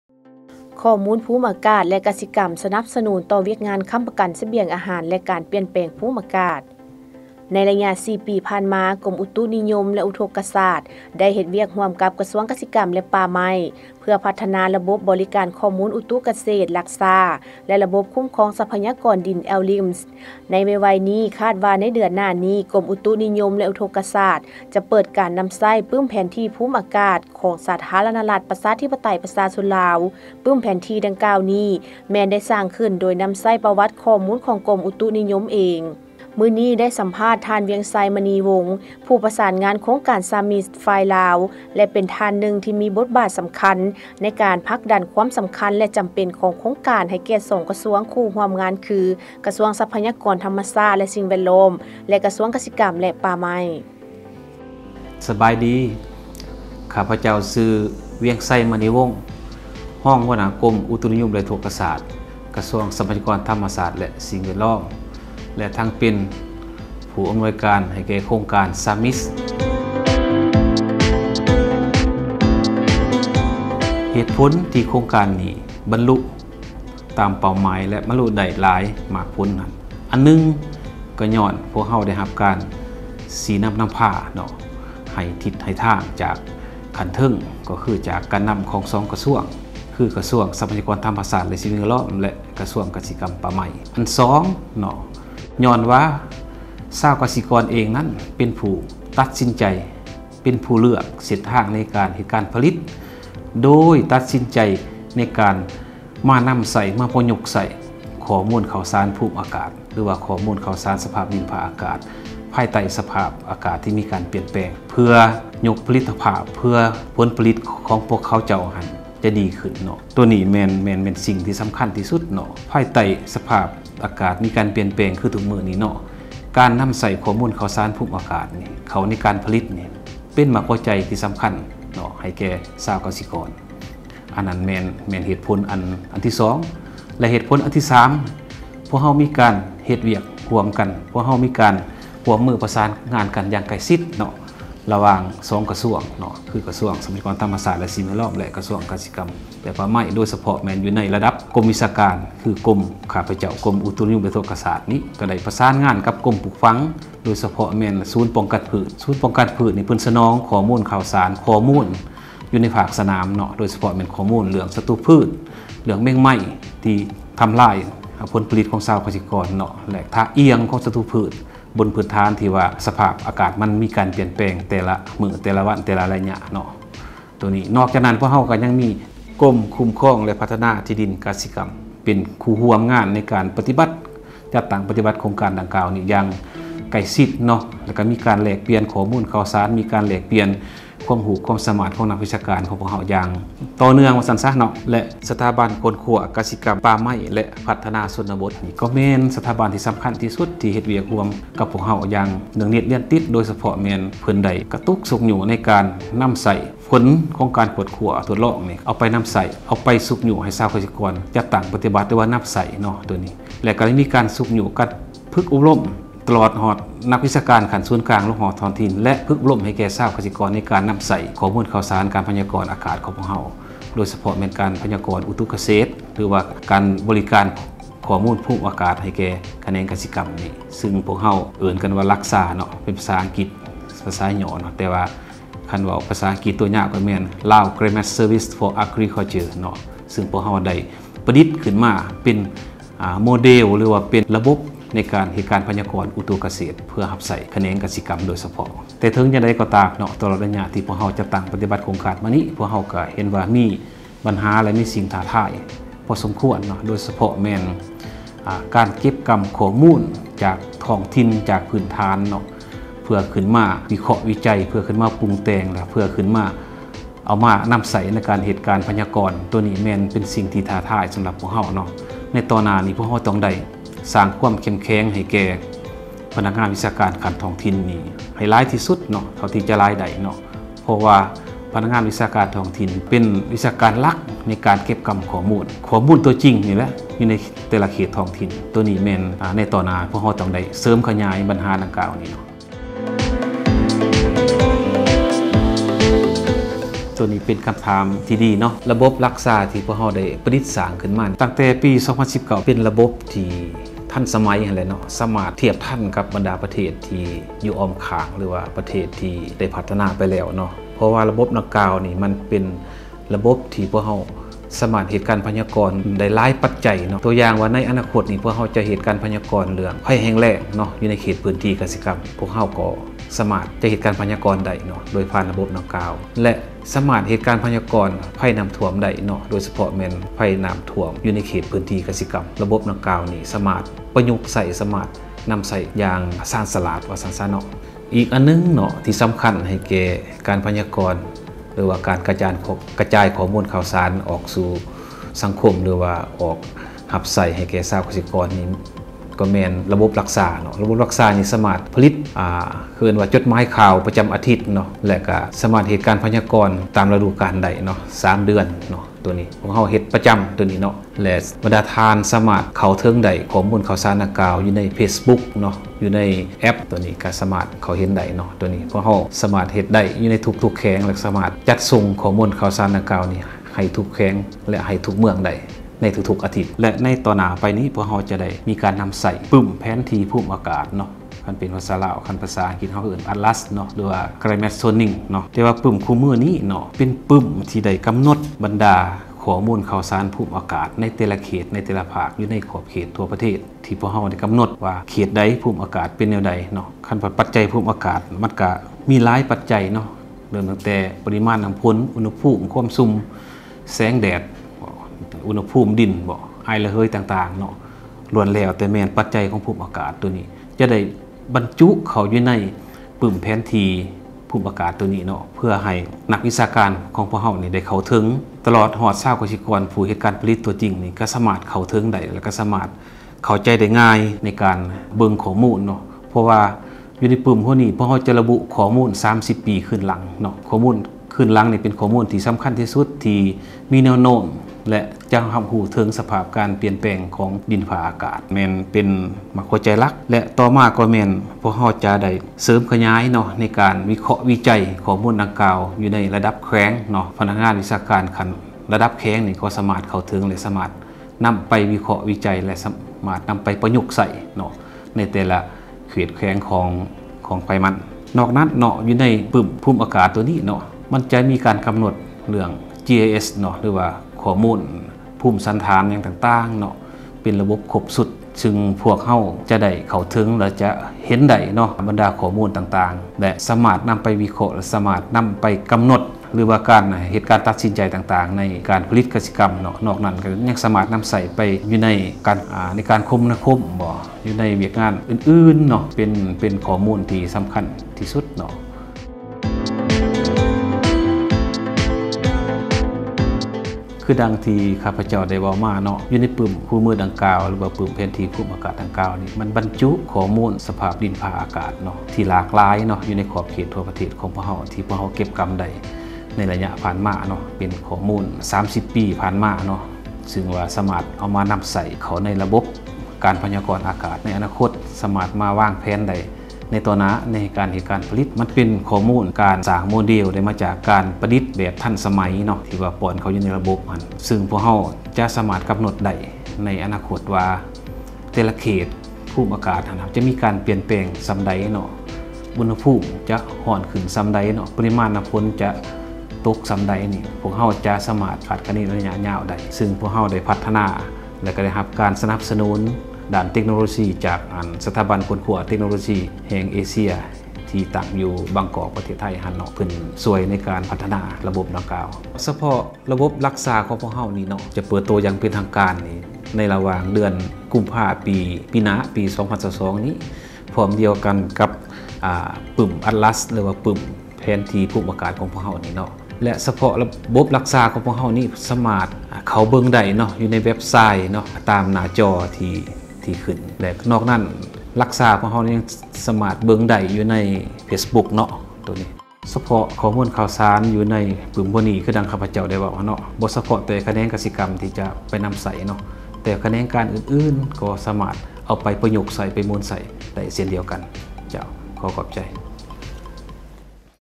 ຂໍ້​ມູນ​ພູມ​ອາ​ກາດ ແລະ ກະ​ສິ​ກຳ
ຂໍ້​ມູນ​ພູມ​ອາ​ກາດ ແລະ ກະ​ສິ​ກຳ ສະ​ໜັບ​ສະ​ໜູນ​ຕໍ່​ວຽກ​ງານ ຄ້ຳ​ປະ​ກັນ​ສະ​ບຽງ​ອາ​ຫານ ແລະ ການ​ປ່ຽນ​ແປງ​ພູມ​ອາ​ກາດ